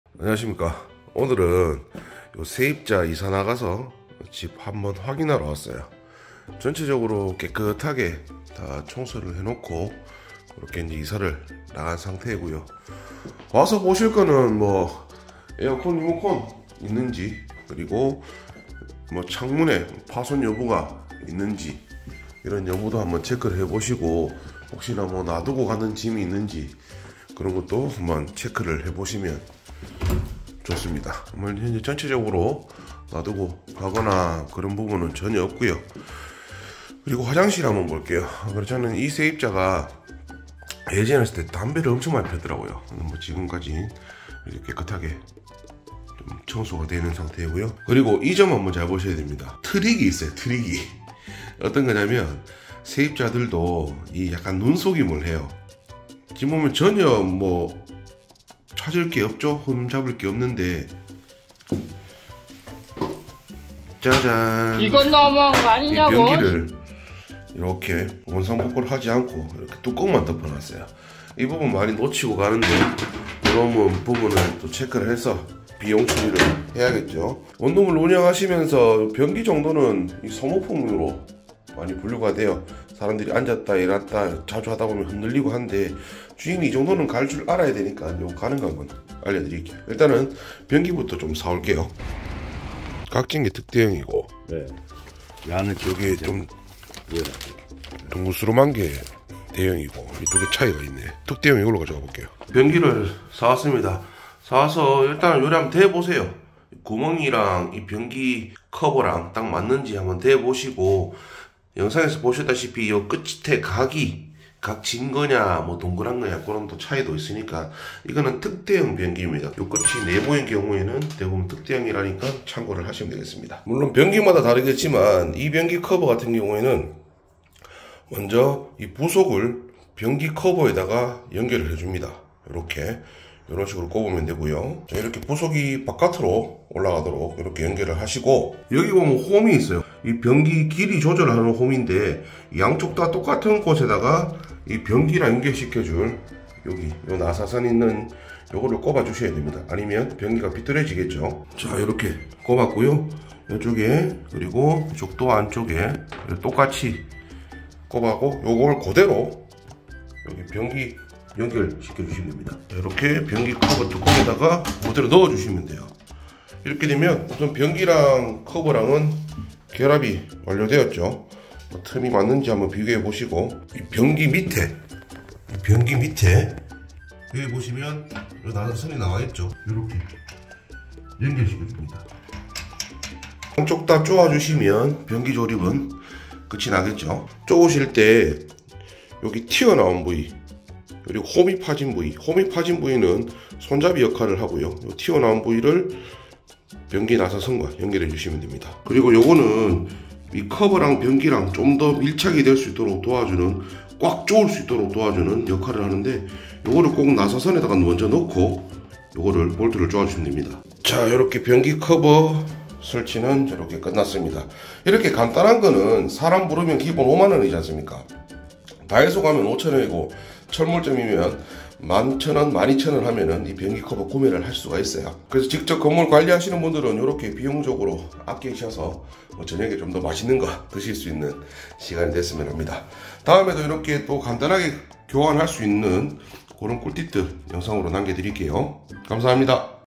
건물 관리할라면 변기뚜껑은 교체 할 줄 알아야지
세입자가 눈속임으로 가리고 간 경우 많아요~
꼼꼼히 보시고 간단하게 저비용으로 교체하세요

변기커버 교체는 쉽습니다^^